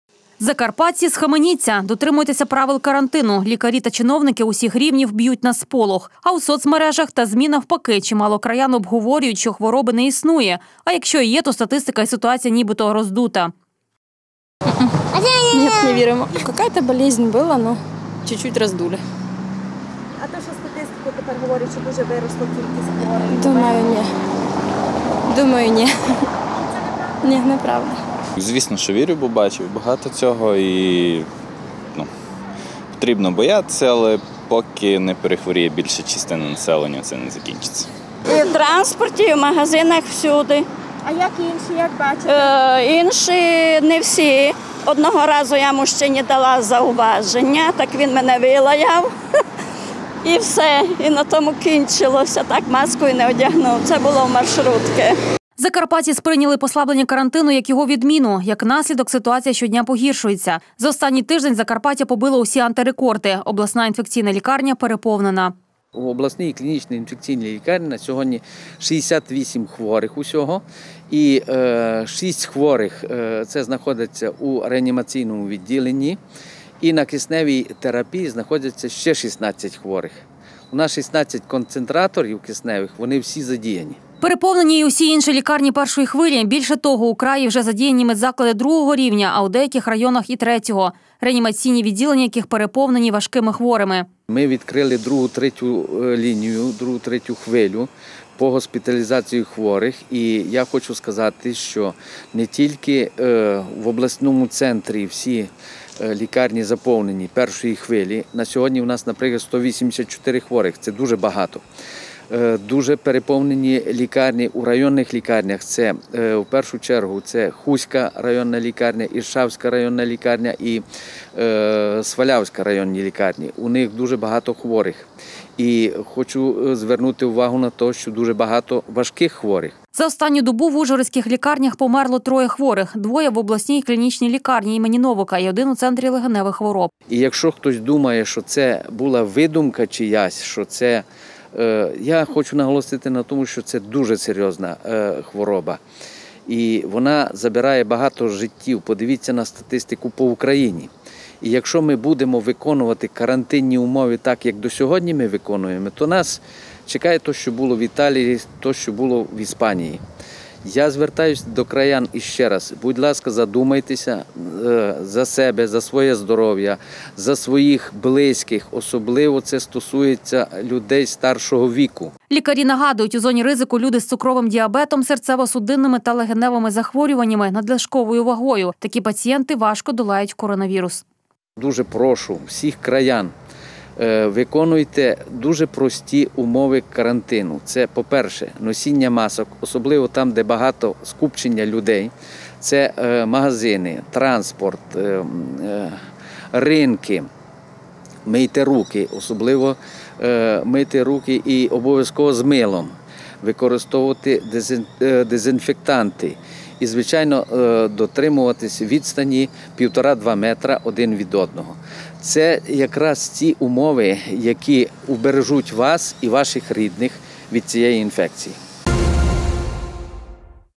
Погіршення ситуації
Закарпатці, схаменіться! Дотримуйтеся правил карантину –лікарі та чиновники усіх рівнів б’ють на сполох, а у соцмережах і ЗМІ – навпаки, чимало краян обговорюють, що хвороби не існує, а якщо і є, то статистика і ситуація, ніби- то роздута. Закараптці сприйняли послаблення карантину як його відміну. Як наслідок, ситуація щодня погіршується. За останній тиждень Закарпаття побило усі антирекорди. Обласна інфекційна лікарня – переповнена.
Переповнені й усі інші лікарні І хвилі, більше того у краї вже задіяні  медзаклади ІІ рівня, а у деяких районах і ІІІ. реанімаційні відділення яких переповнені важкими хворими. За останню добу в Ужгородських лікарнях померло 3 хворих: двоє у обласній клінічній лікарні імені Новака і один у Центрі легеневих хвороб. Лікарі нагадують, у зоні ризику люди з цукровим діабетом, серцево –судинними і легеневими захворюваннями, надлишковою вагою. Такі пацієнти важко долають коронавірус.